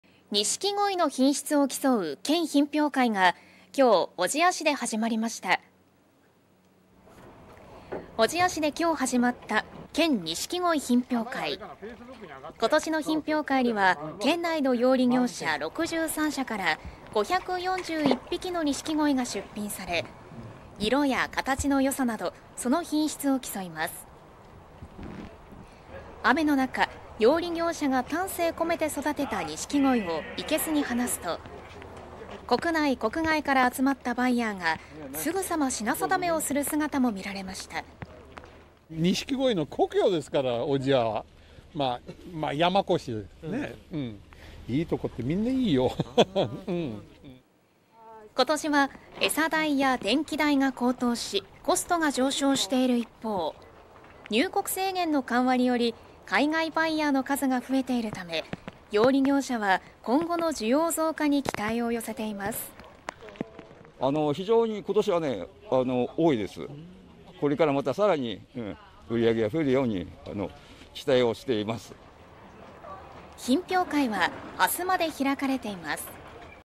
泳ぐ宝石”ニシキゴイ”の品評会に海外バイヤーの姿！養鯉業者から期待の声【新潟・小千谷市】 (22/11/04 12:13)
ニシキゴイの品質を競う県品評会が４日、小千谷市で始まりました。

小千谷市で４日に始まった県錦鯉品評会。今年の品評会には県内の養鯉業者「６３社」から「５４１匹」の錦鯉が出品され、色や形の良さなどその品質を競います。

雨の中、養鯉業者が丹精込めて育てたニシキゴイをいけすに放すと、国内国外から集まったバイヤーがすぐさま品定めをする姿も見られました。

【ハワイから来た愛好家は】
「錦鯉の故郷ですから小千谷は。みんないい鯉だよ」

今年はエサ代や電気代が高騰しコストが上昇している一方、入国制限の緩和により海外バイヤーの数が増えているため、養鯉業者は今後の需要増加に期待を寄せています。

【県錦鯉品評会　間野等副実行委員長】
「（海外バイヤー）今年は非常に多い。これからまたさらに売り上げが上がるよう期待している」

県錦鯉品評会は５日まで開かれています。